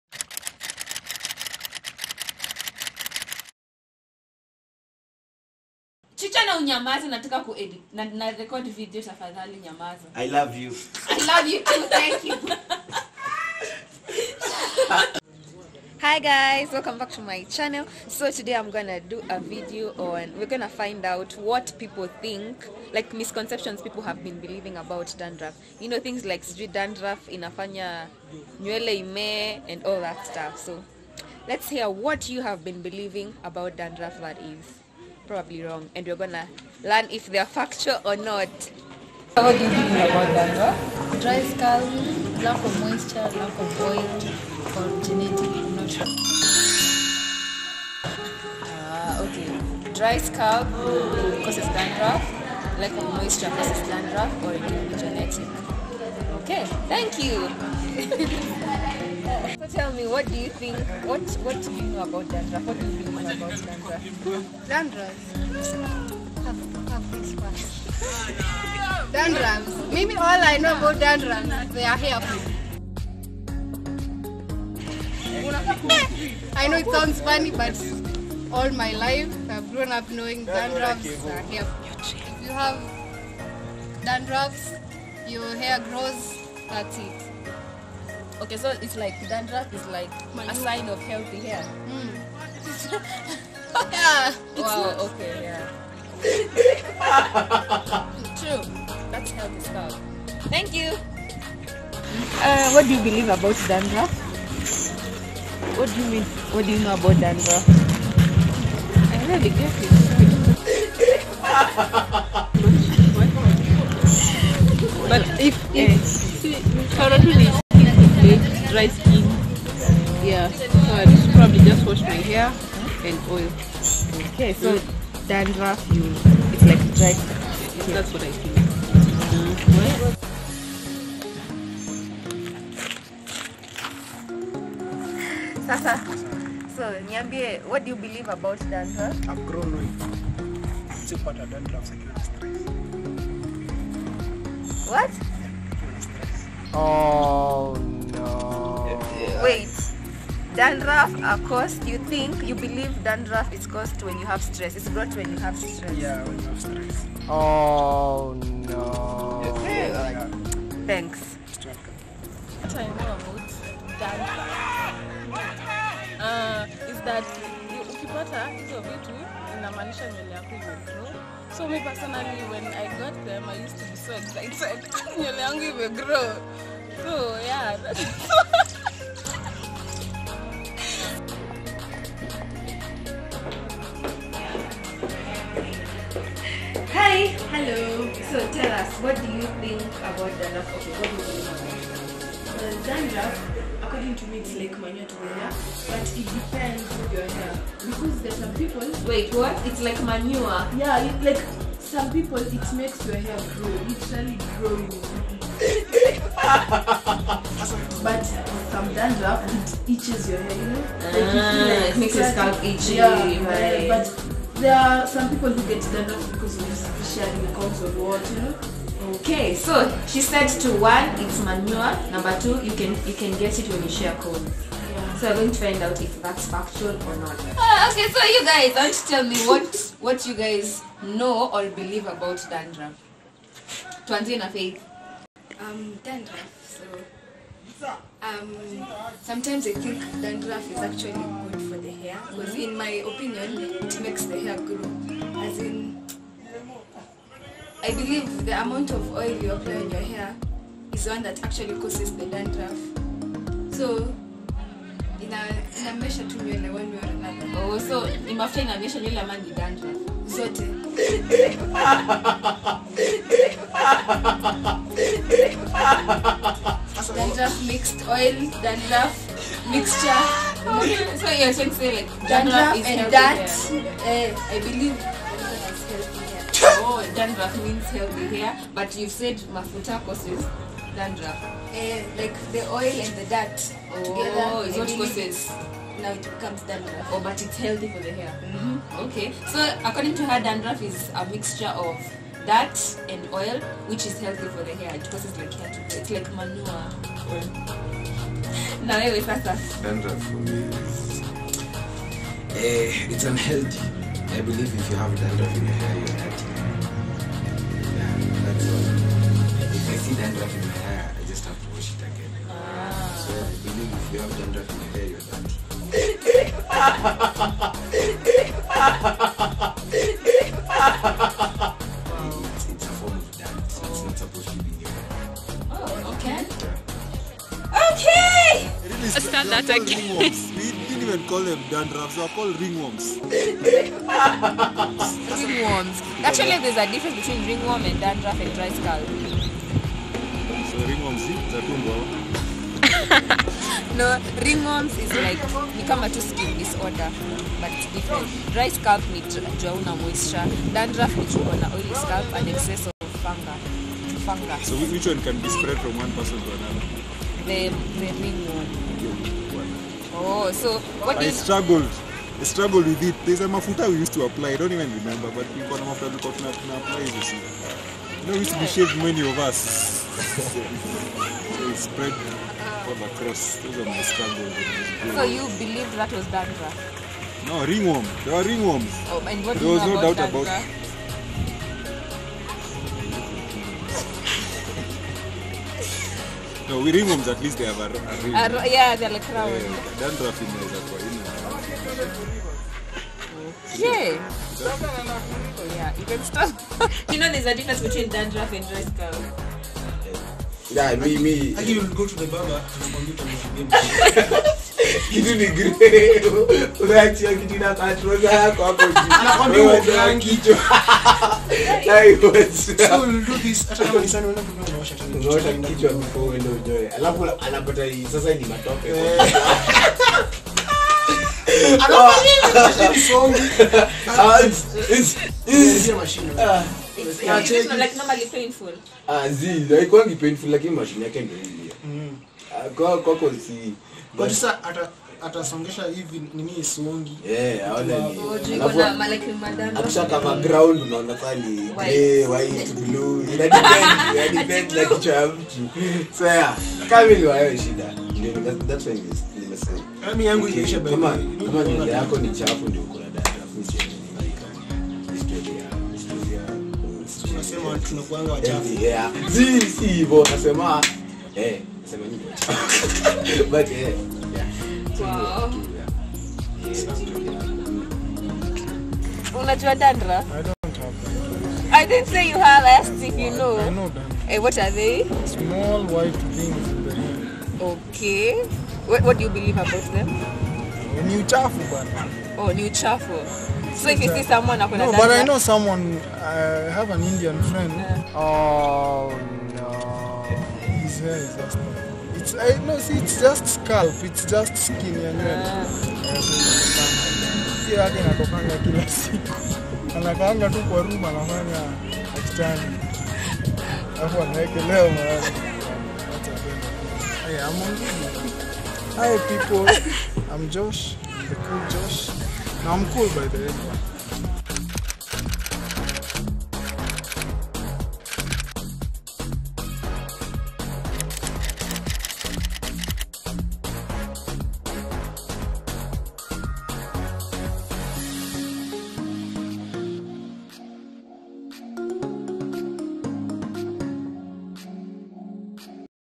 what actually causes dandruff
hey people
This week's video is just super hilarious.
The ideas, opinions, misconceptions people have about dandruff.
Share with us down the comments below what you had been believing about dandruff.
love
Naureiness.